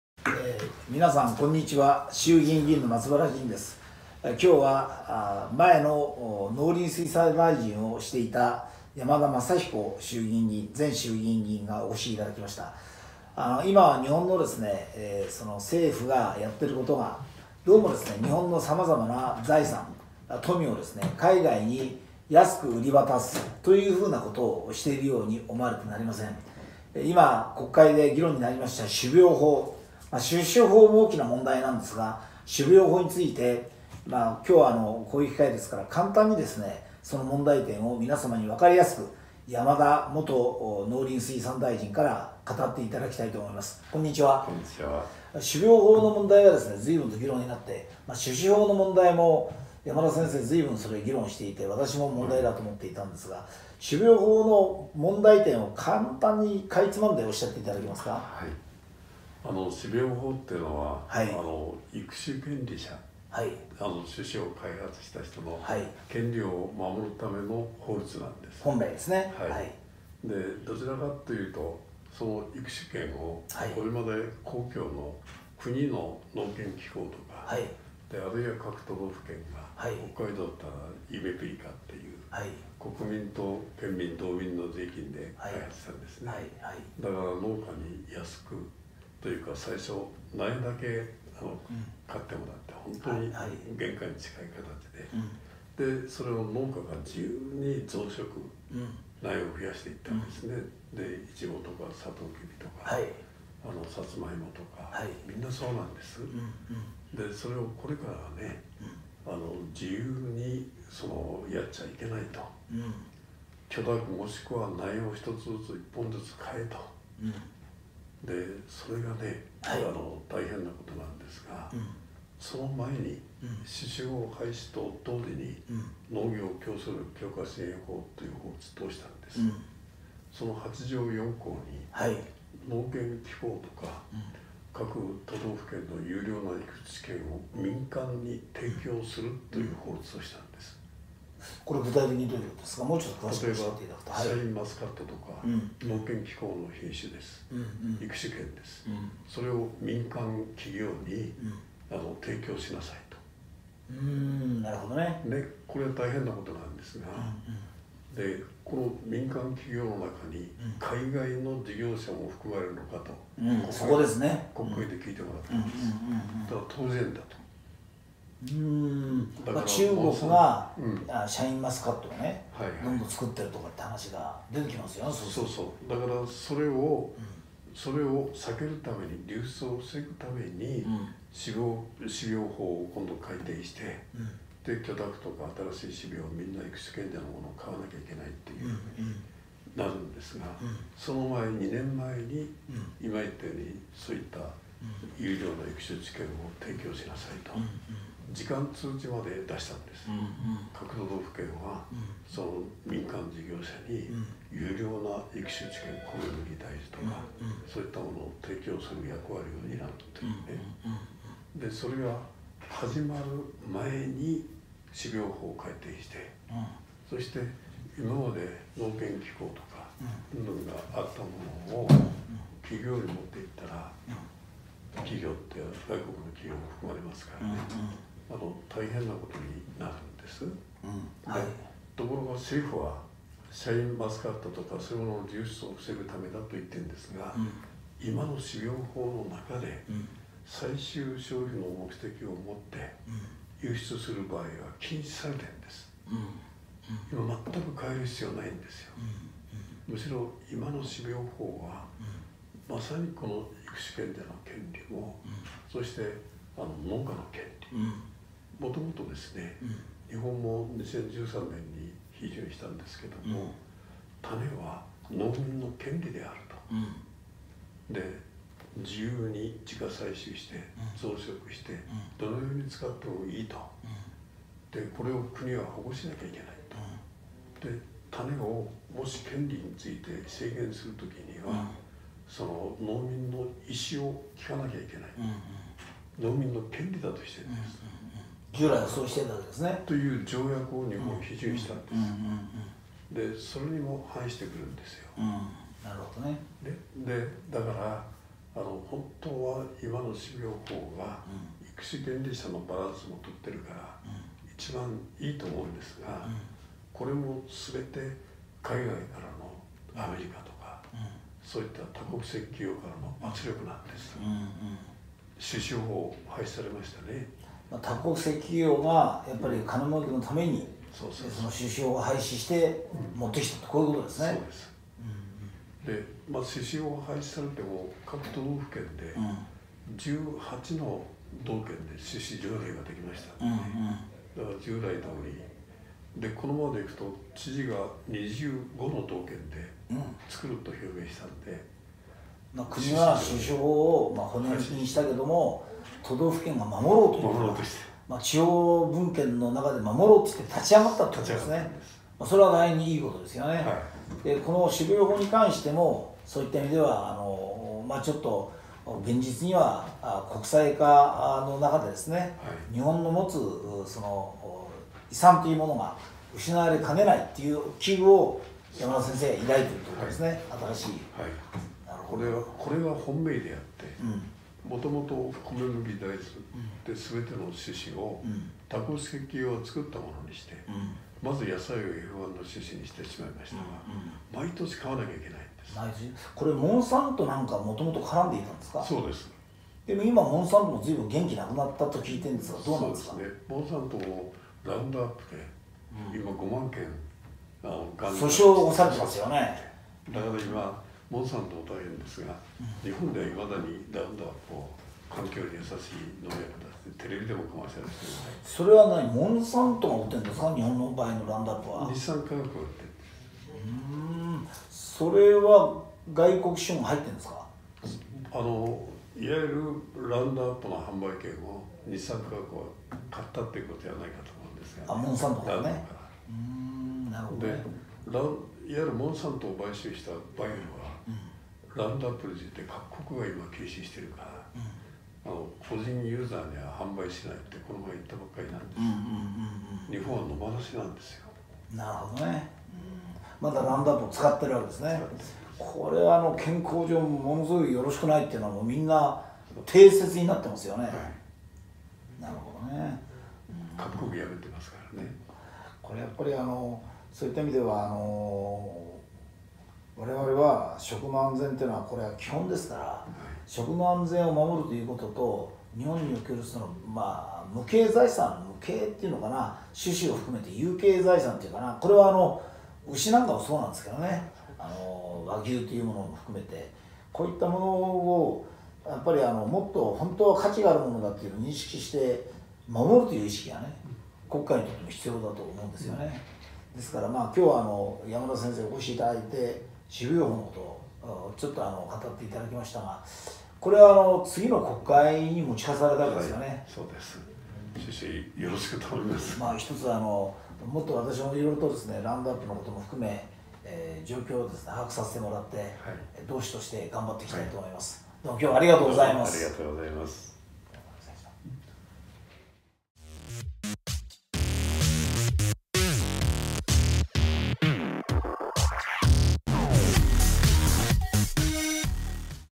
【山田正彦に聞く】秋国会の争点『種苗法』改正案の持つ意味「農家の声は？育種家の利益？消費者にデメリット？」 衆議院議員 まつばら仁 （無所属）
山田正彦 元農林水産大臣